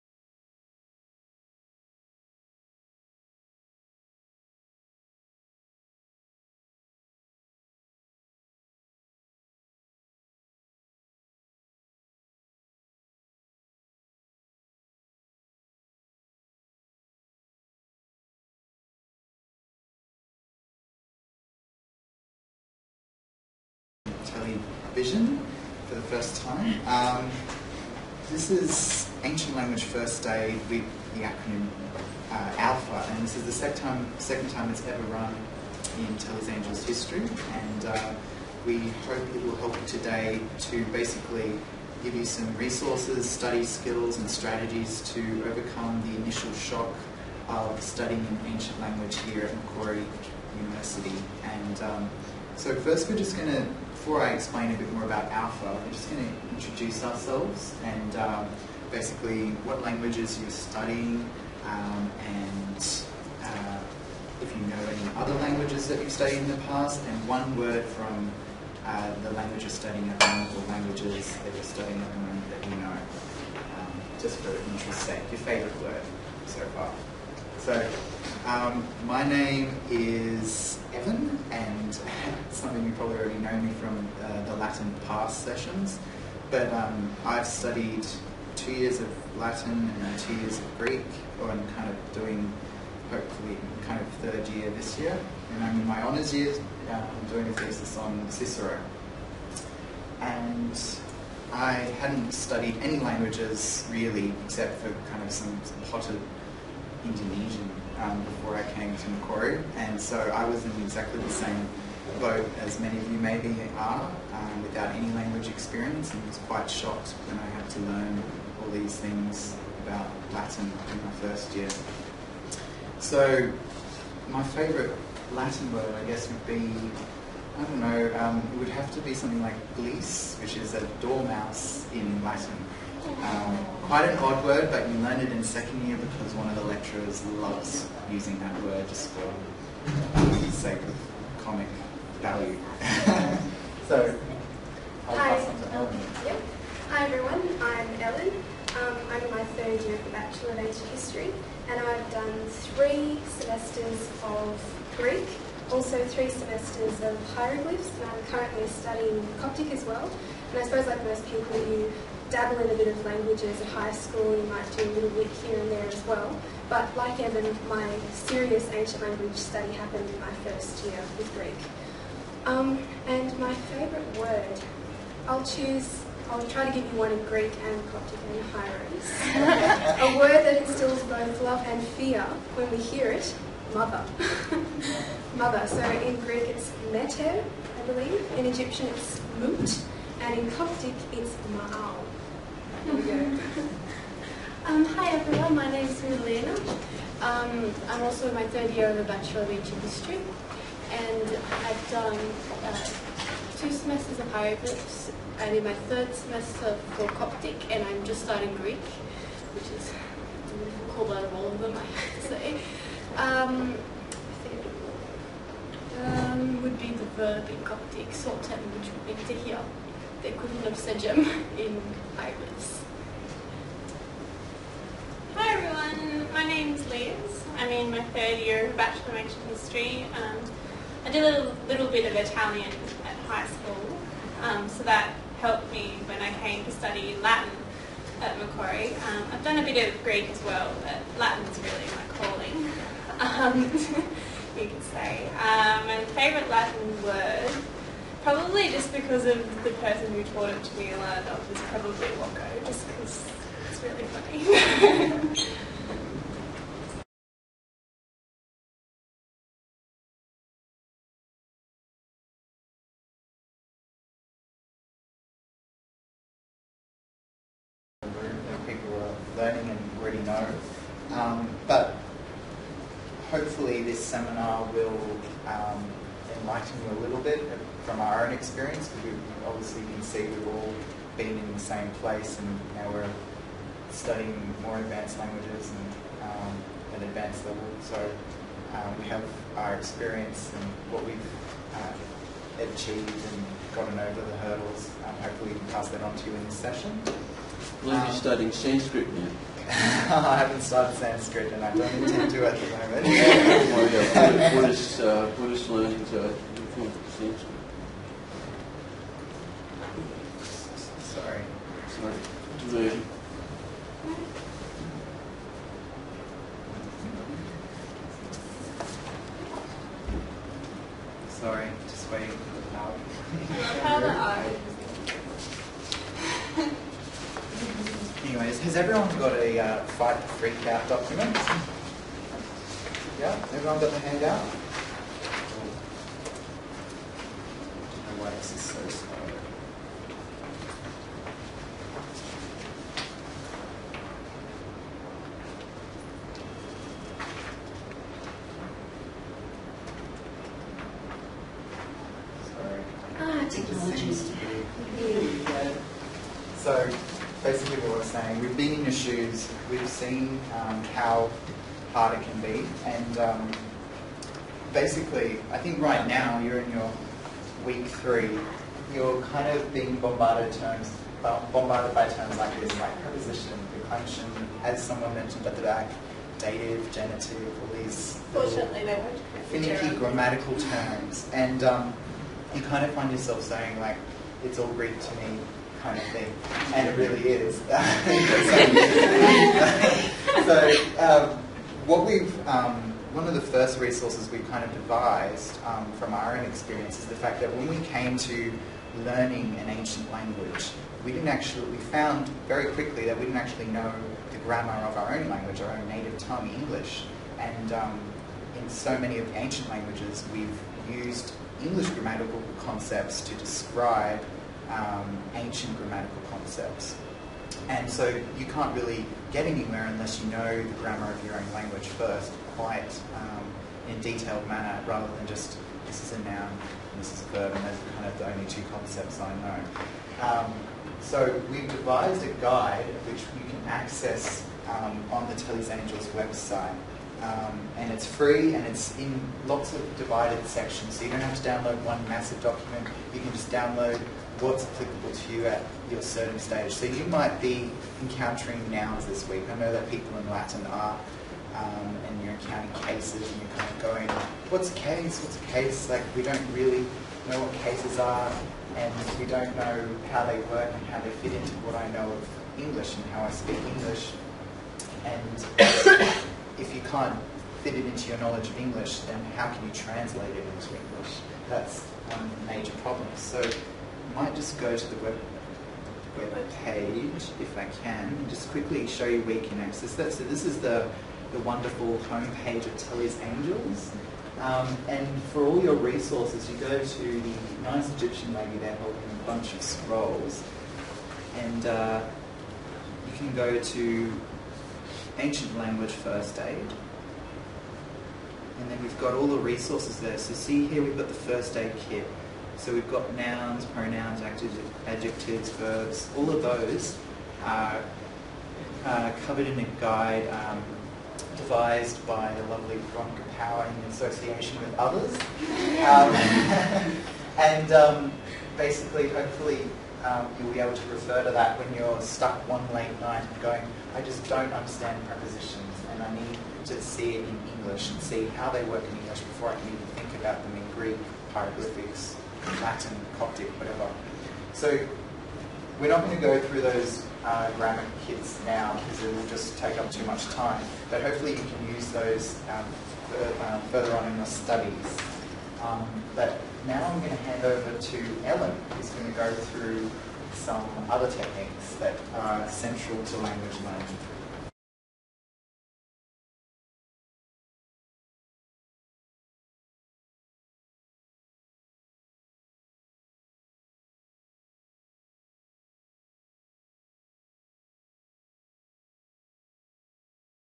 ALFA Seminar Part 1 of 5
In this section of the Ancient Language First Aid Seminar, Tele's Angels Mentors introduce themselves and provide some resources to aid in the understanding of grammatical concepts

This link will take you directly to our ALFA resources, all of which are available to download